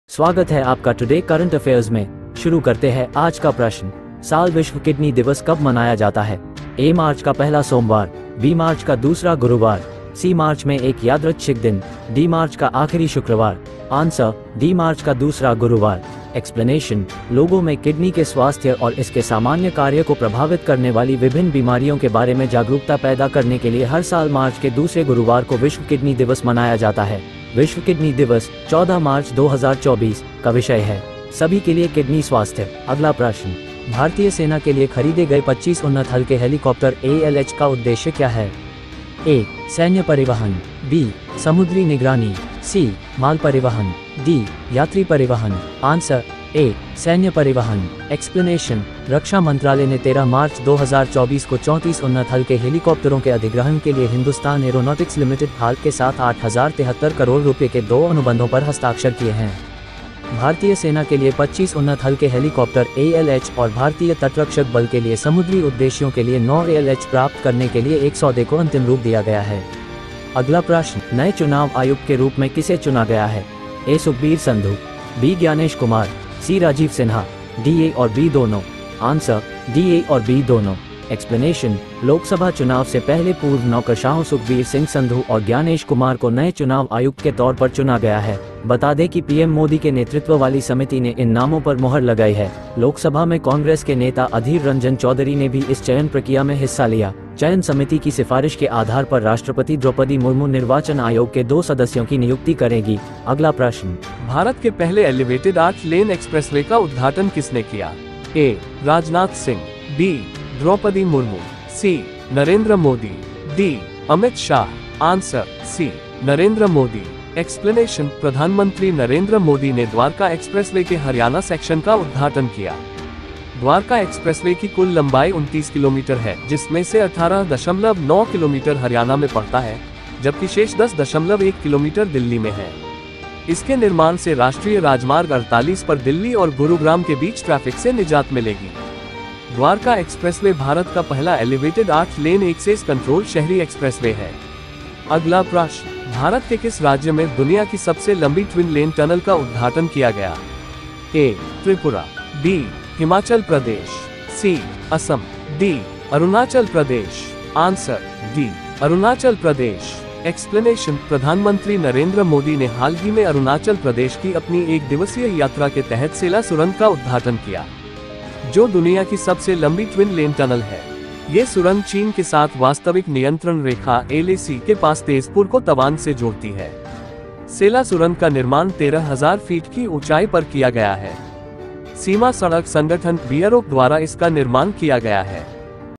15 March 2024 |Current Affairs Today |भारत में दुनिया की सबसे लंबी ट्विन-लेन टनल| विश्व किडनी दिवस
15 March 2024 |Current Affairs Today |भारत में दुनिया की सबसे लंबी ट्विन-लेन टनल| विश्व किडनी दिवस

15 March 2024 |Current Affairs Today |Daily Current Affairs In Hindi &English|Current affair 2024

1. भारत के किस राज्य में दुनिया की सबसे लंबी ट्विन-लेन टनल का उद्घाटन किया गया?- अरुणाचल प्रदेश
2. भारत के पहले एलिवेटेड 8 लेन एक्सप्रेसवे का उद्घाटन किसने किया?- नरेंद्र मोदी
3. चुनाव आयुक्त के रूप में किसे चुना गया है?- सुखबीर संधु , ज्ञानेश कुमार
4. भारतीय सेना के लिए खरीदे गए 25 उन्नत हल्के हेलीकॉप्टर (ALH) का उद्देश्य क्या है?- सैन्य परिवहन
5. साल में विश्व किडनी दिवस कब मनाया जाता है?- मार्च का दूसरा गुरुवार

dwarka expressway,
india's first 8 lane elevated road,